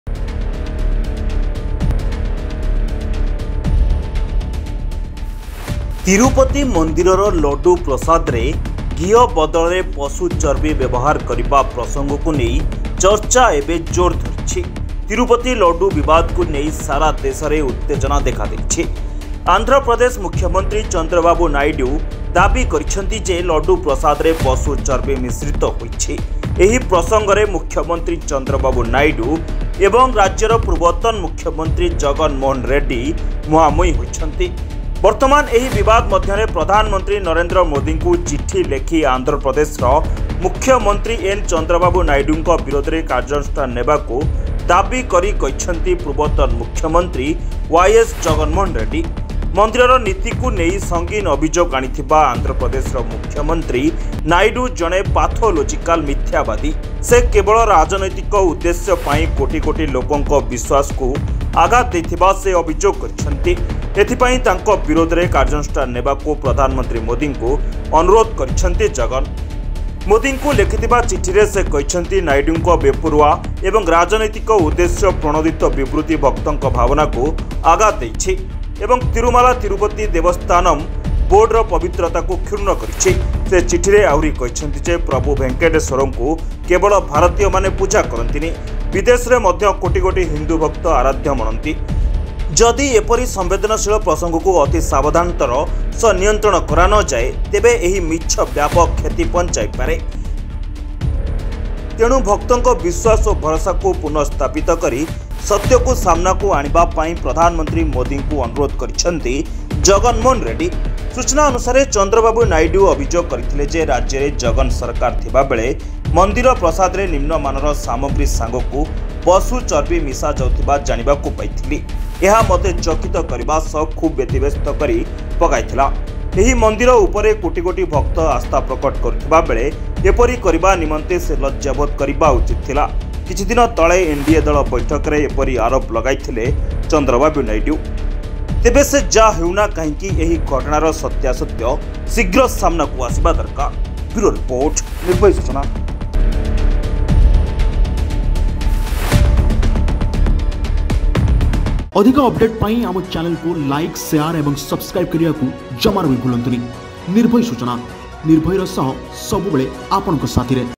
ନାଇଡ଼ୁଙ୍କ ବିରୋଧରେ ଚିଠି ଲେଖିଲେ ଜଗନ୍‌, ମୋଦି ନେବେ କି ଆକ୍ସନ ? | Nirbhay Suchana
ତିରୁପତି ମନ୍ଦିରର ଲଡୁ ପ୍ରସାଦରେ ଘିଅ ବଦଳରେ ପଶୁ ଚର୍ବି ବ୍ୟବହାର କରିବା ପ୍ରସଙ୍ଗକୁ ନେଇ ଚର୍ଚ୍ଚା ଜୋର ଧରିଛି। ତିରୁପତି ଲଡୁ ବିବାଦକୁ ନେଇ ସାରା ଦେଶରେ ଉତ୍ତେଜନା ଦେଖାଦେଇଛି। ଆନ୍ଧ୍ରପ୍ରଦେଶ ମୁଖ୍ୟମନ୍ତ୍ରୀ ଚନ୍ଦ୍ରାବାବୁ ନାଇଡୁ ଦାବି କରିଛନ୍ତି ଯେ ଲଡୁ ପ୍ରସାଦରେ ପଶୁ ଚର୍ବି ମିଶ୍ରିତ ହୋଇଛି। ଏହି ପ୍ରସଙ୍ଗରେ ମୁଖ୍ୟମନ୍ତ୍ରୀ ଚନ୍ଦ୍ରାବାବୁ ନାଇଡୁ ଏବଂ ପୂର୍ବତନ ମୁଖ୍ୟମନ୍ତ୍ରୀ ଜଗନ ରେଡ୍ଡୀ ମୁହାଁମୁହିଁ ହୋଇଛନ୍ତି।